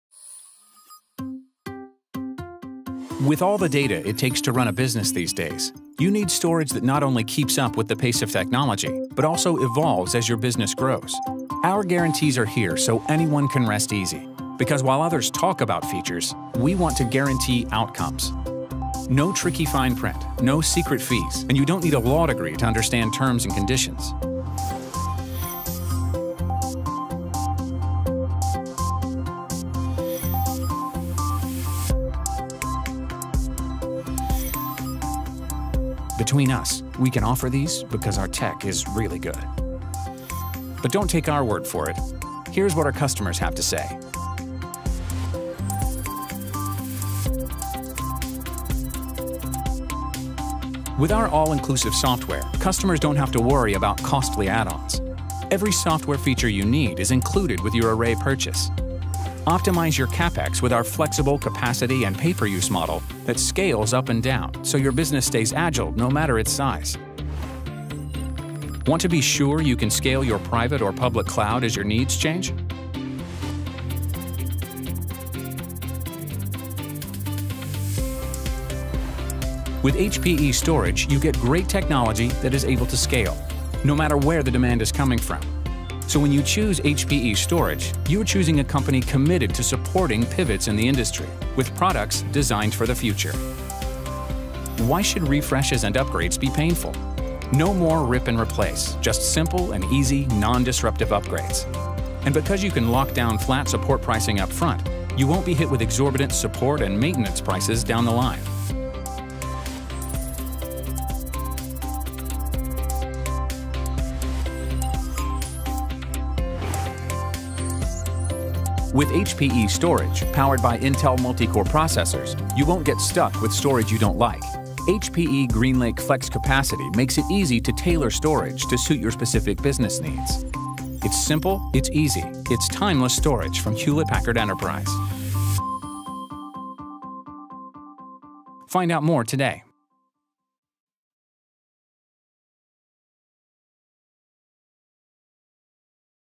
HPE Storage is timeless
Looking for data storage systems that keep up and evolve as your buseiness grows? HPE 3PAR guarantees outcomes and provides all-inclusive software, scalable cloud, flexible consumption, and futureproof technology.